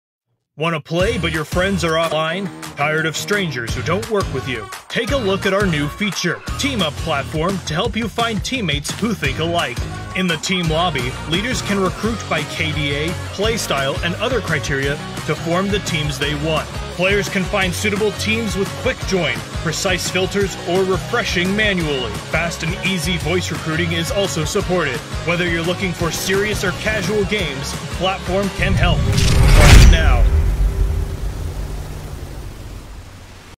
Want to play, but your friends offline.
Want to play, but your friends offline? Tired of strangers who don't work with you? Take a look at our new feature, Team-up Platform.